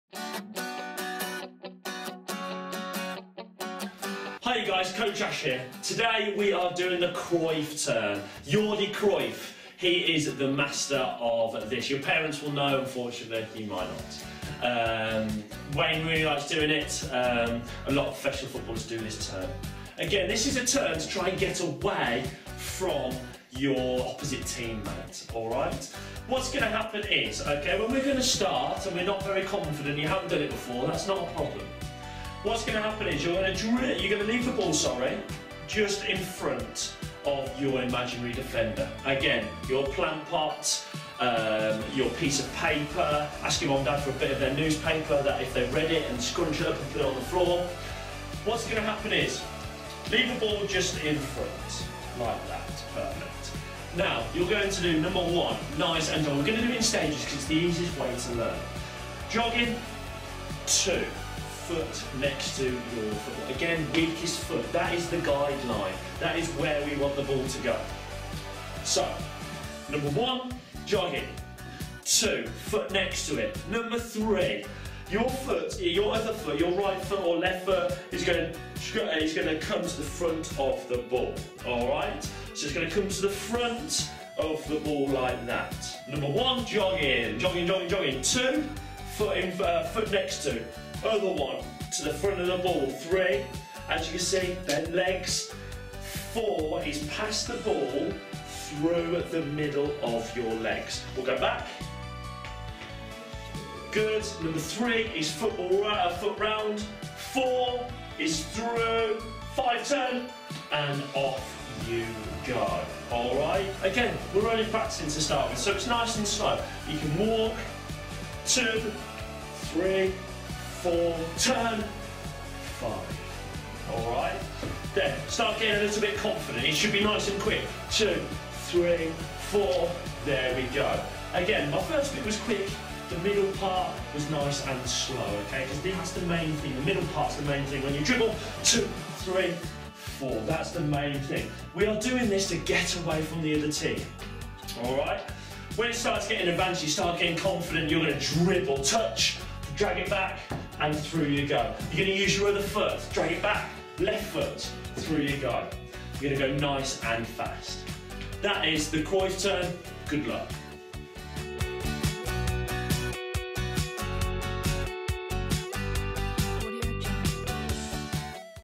FootieBugs Football Skills - The Cruyff Turn
Check out FootieBugs Football Skills Coach Ash!

In this video we'll be looking at the famous Cruyff Turn, created by dutch legend Johan Cruyff! a great skill to show off to your friends!